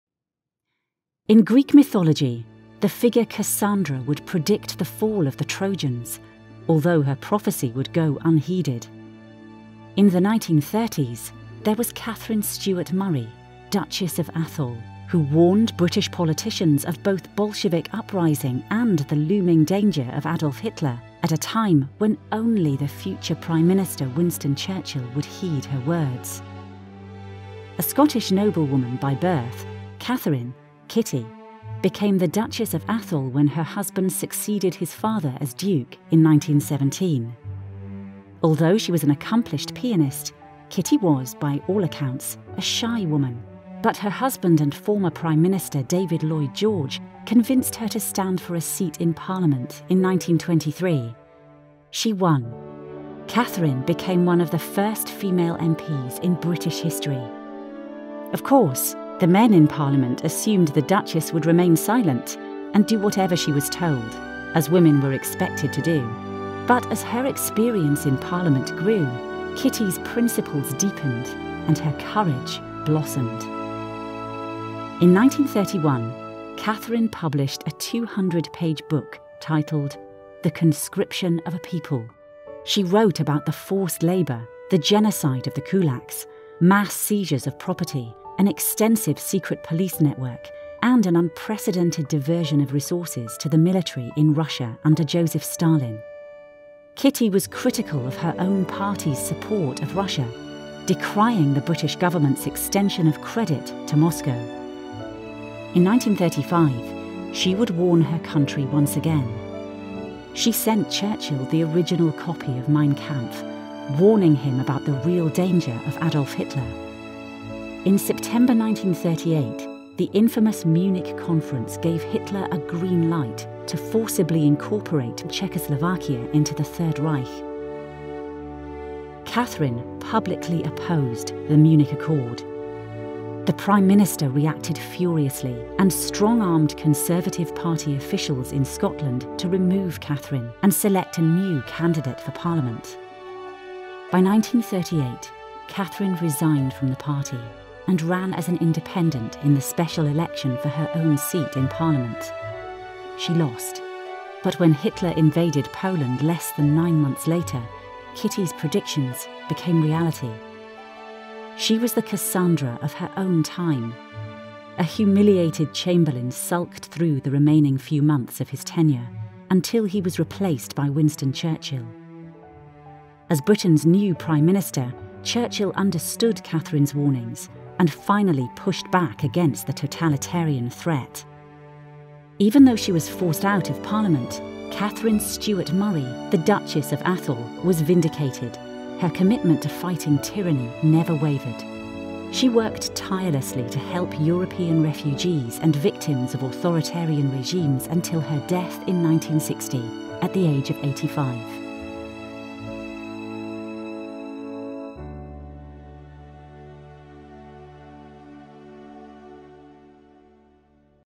The Woman Who Warned the World
As one of the first women to serve in the British Parliament, Katharine Stewart-Murray openly condemned Josef Stalin and the atrocities committed by his Communist regime when other British leaders turned a blind eye. She spoke out critically about domestic policies in the UK that she derided as having “national socialist tendencies.” She sounded the alarm about Adolf Hitler, warning of his violent plans for invasion and genocide, while others were content with appeasing the Nazi dictator. She did what was right despite the consequences.

CREDITS:
Produced by Sean W. Malone
Written by Jaye Davidson & Sean W. Malone, based on a story from Lawrence Reed's book "Real Heroes".
Animated by Tomasz Kaye

Note: Reuploaded to fix error in video.